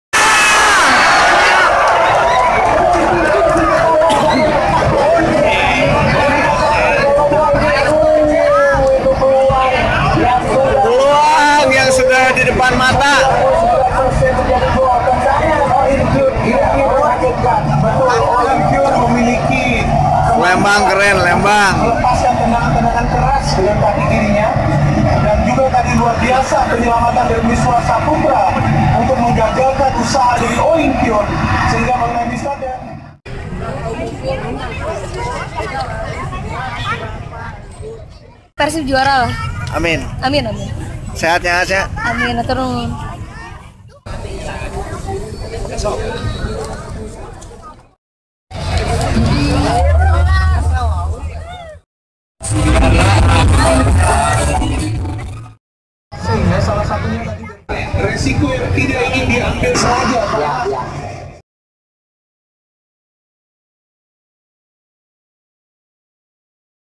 Keren pisan!!! nobar Persib Bandung vs Persebaya di Lembang
Rame banget..nobar Persib Bandung vs Persebaya Surabaya yang di sponsori oleh kopi ABC ini sangat meriah dan menghibur..terlebih Persib berhasil memenangkan pertandingan dengan skor 4 - 3..juara paruh musim euy..sangat membahagiakan Bobotoh..